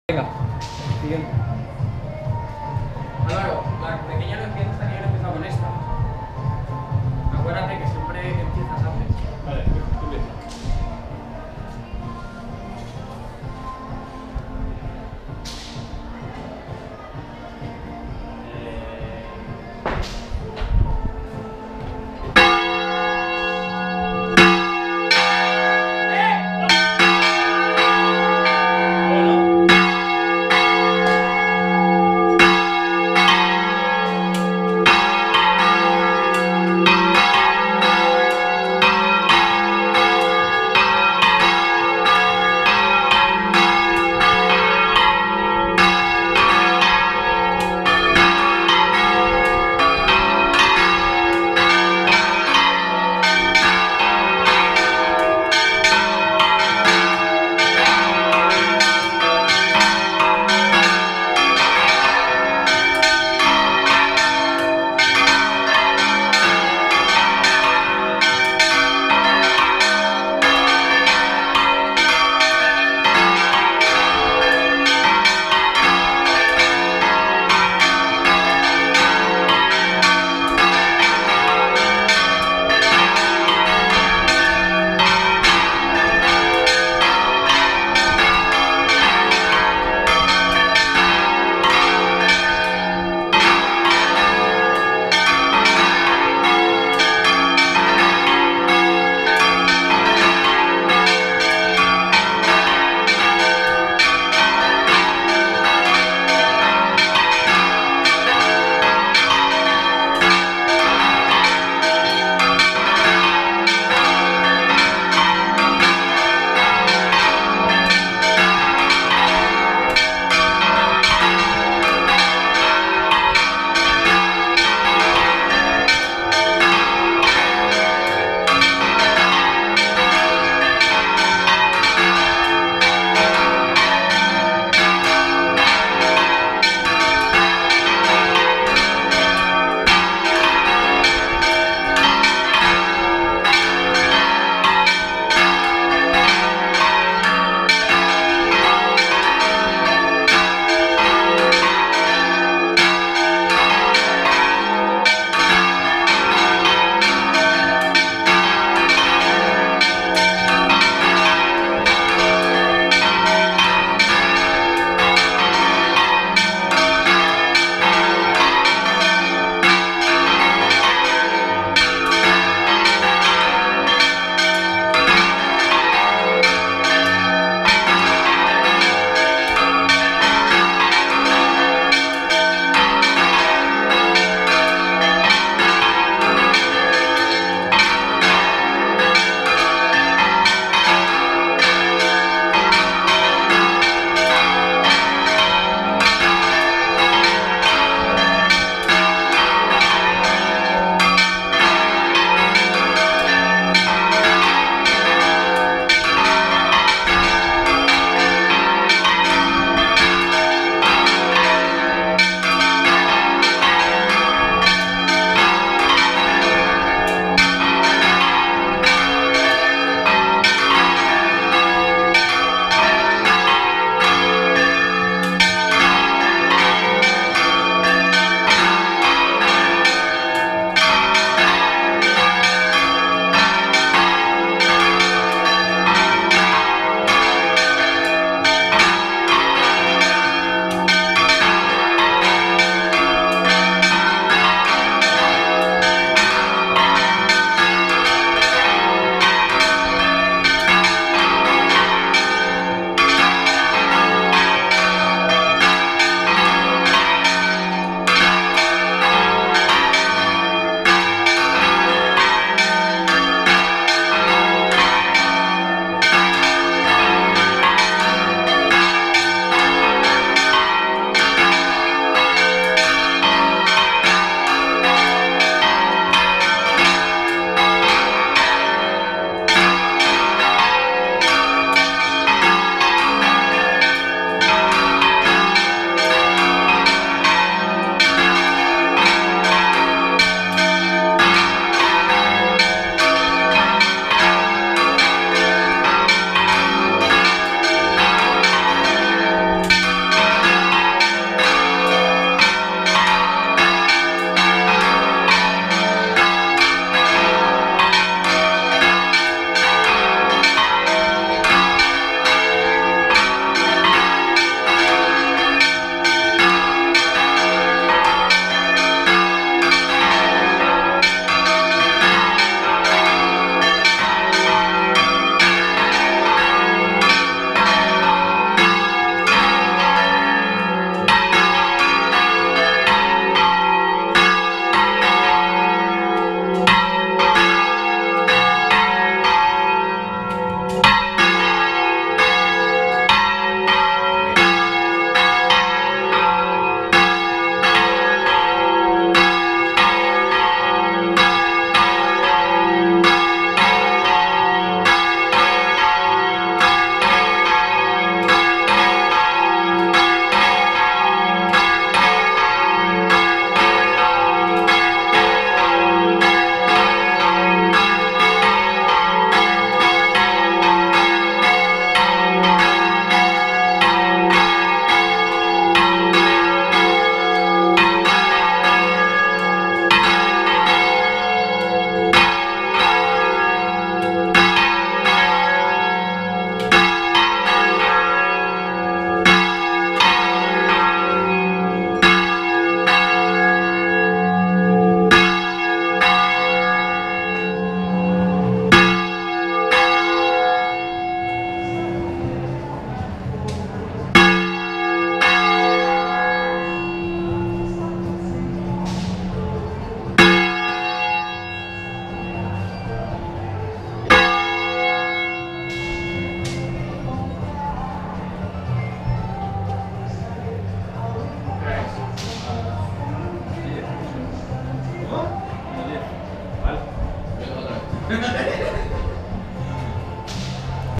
Volteo general de campanas en Valdetorres 2019 // Fiestas septiembre 2019
Tercer Volteo general de campanas para la procesion del Cristo en las fiestas de septiembre de 2019